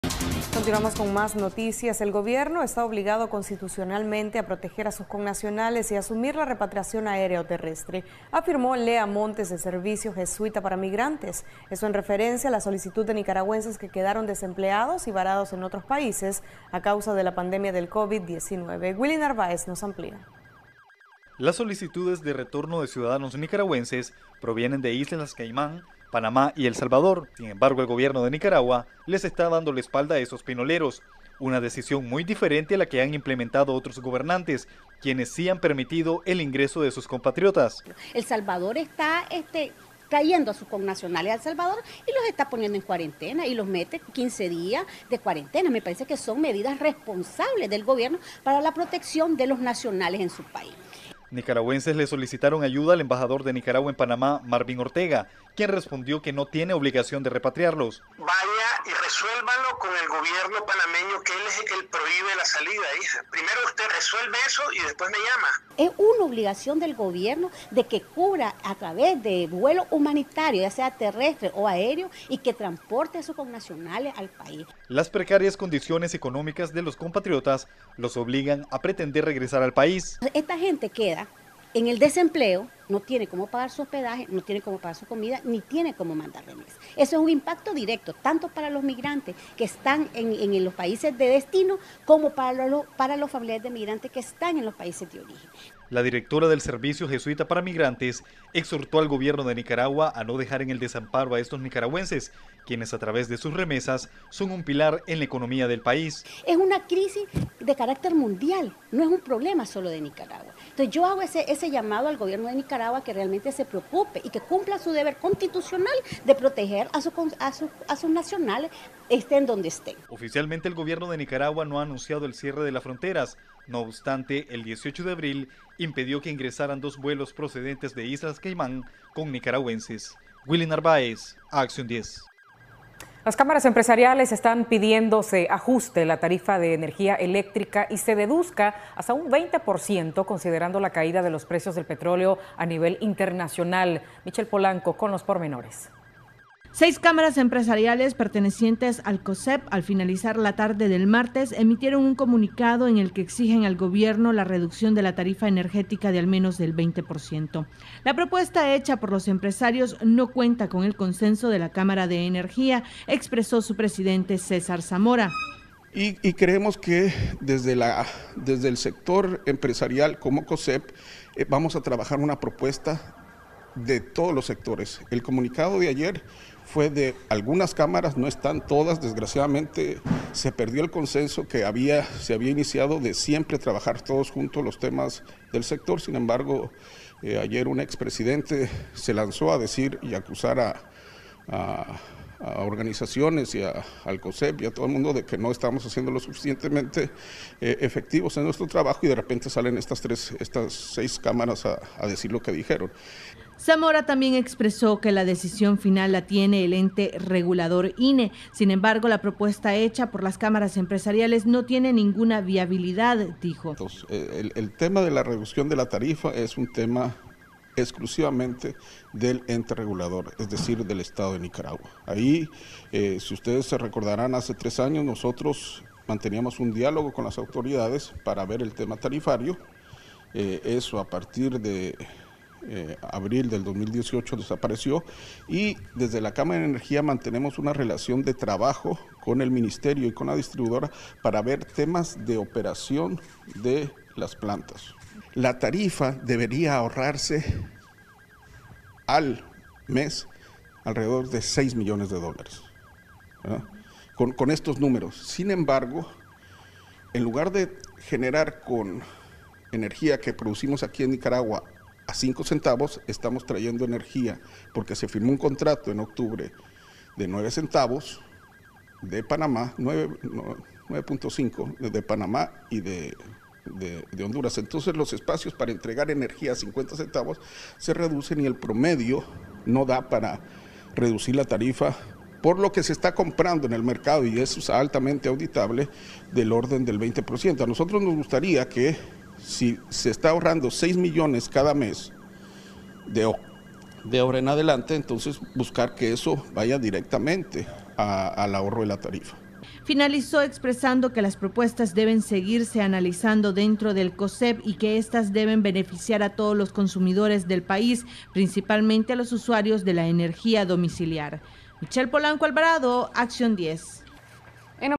El Gobierno esta en la obligación de repatriar a sus connacionales
El gobierno esta obligado constitucionalmente a proteger a sus connacionales y asumir la repatriación aérea o terrestre afirmó Lea Montes del servicio jesuita para migrantes en referencia a la solicitud de Nicaragüenses que quedaron desempleados y varados en otros países.